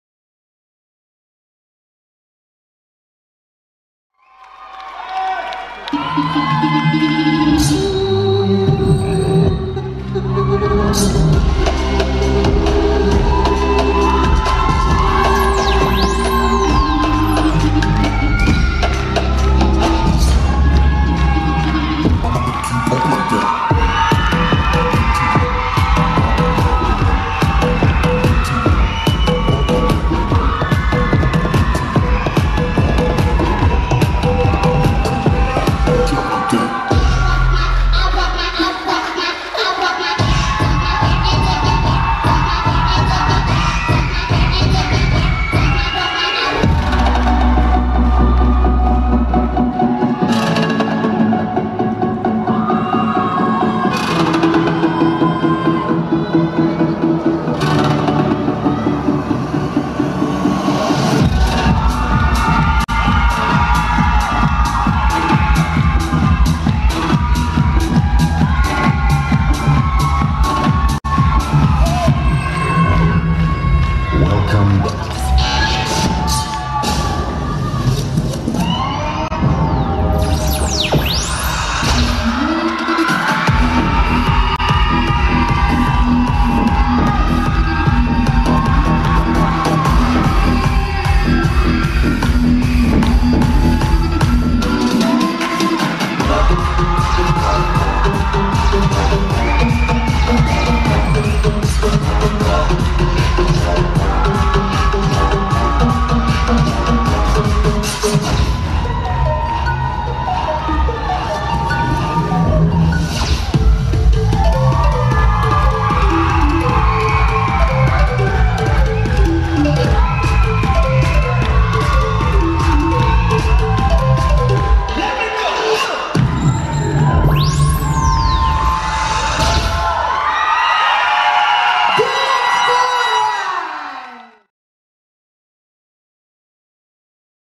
TŠ Dance4Life - Touch the universe
MČR CDO 2. místo
hobby disco dance - formace - děti
r. 2024